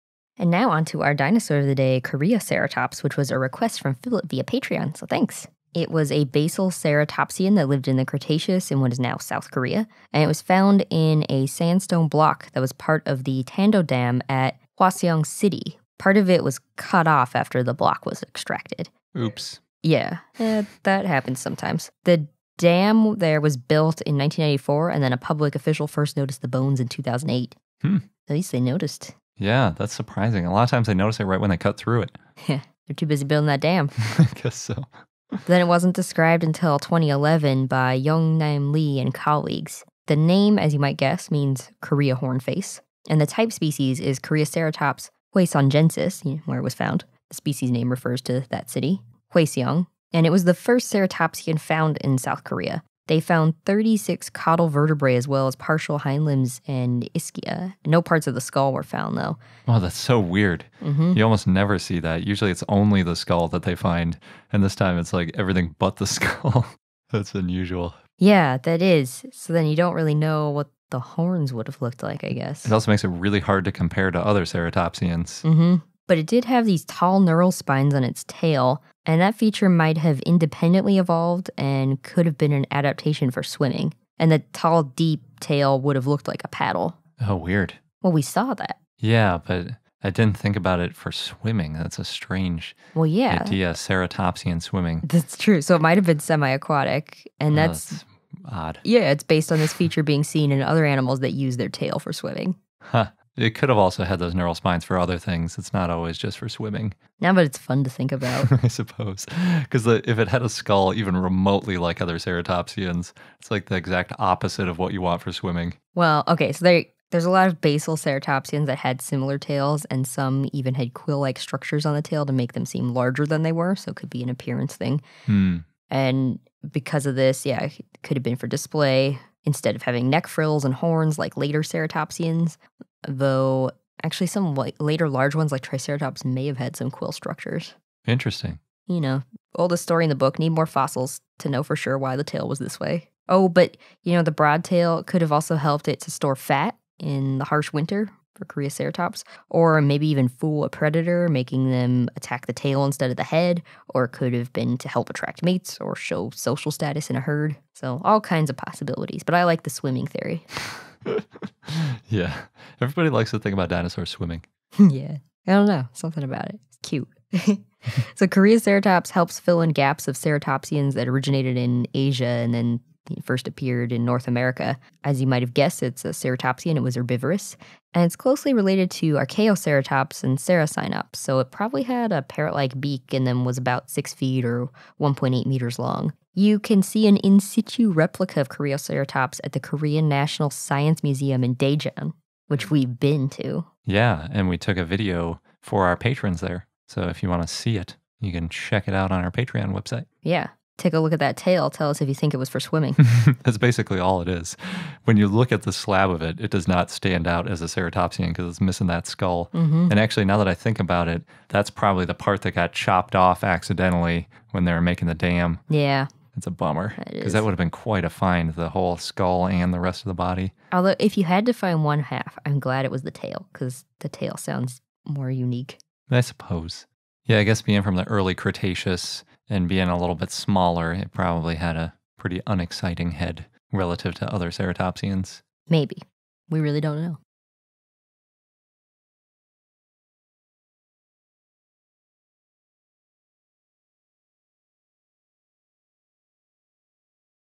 Koreaceratops: Dinosaur of the Day
For the full episode where this dinosaur of the day was originally featured check out

Basal ceratopsian that lived in the Cretaceous in what is now South Korea
Found in a sandstone block that was part of the Tando dam at Hwaseong City (part of it cut off, after the block was extracted)
Dam was built in 1994, and a public official first noticed the bones in 2008
Described in 2011 by Yuong-Name Lee and colleagues
Name means "Korea horn face"
Type species is Koreaceratops hwaseongensis
Species name refers to Hwaseong City
First ceratopsian found in South Korea
Found 36 caudal vertebrae, as well as partial hindlimbs and ischia
No parts of the skull were found
Had tall neural spines on its tail, a feature that may have independently evolved and could have been an adaptation for swimming
Tall, deep tail would have looked like a paddle
So, may have been semi-aquatic, based on this feature being seen in other animals that use their tail for swimming
However, many basal ceratopsians had similar tails, and some even had quill-like structures on the tail to make them seem larger than they were
Because of this, basal ceratopsians may have used tail ornamentation for display rather than neck frills and horns like later ceratopsians (though some later large ones, like Triceratops, may have had quill structures)
Need more specimens to know for sure why the tail was that way
Broad tail may have also have stored fat to help Koreaceratops survive in harsh weather, or fool a predator by making them attack the tail instead of the head, or attract mates or show social status in a herd
Koreaceratops helps fill in gaps of ceratopsians originating in Asian and their first appearance in North America
Herbivorous
Closely related to Archaeoceratops and Cerasinops, so it probably had a parrot-like beak and was about 6 ft (1.8 m) long